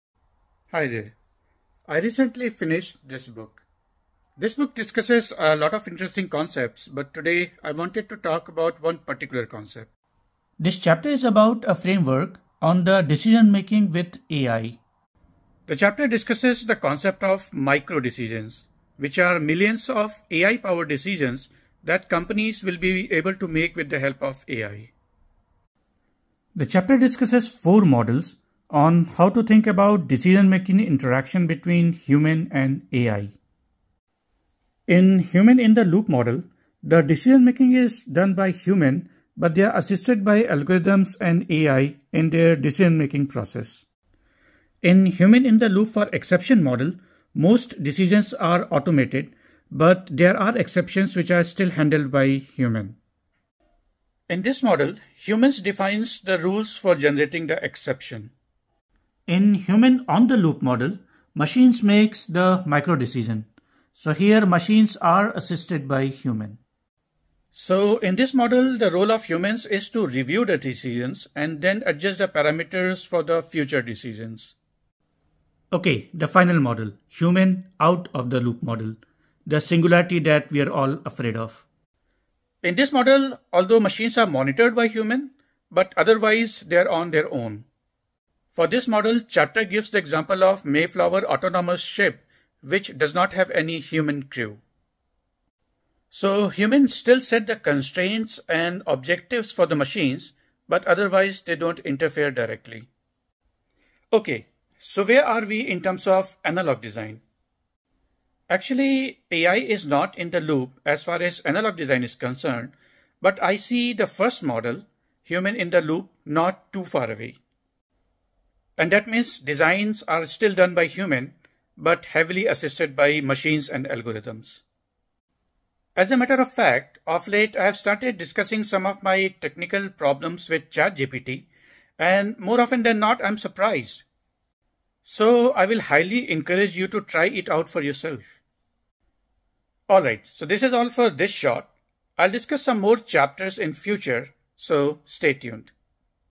8S Decision making with AI
From the book HBR AI basics for managers. AI will impact how we will make the decisions. This chapter provides a useful framework to think about decision-making with AI.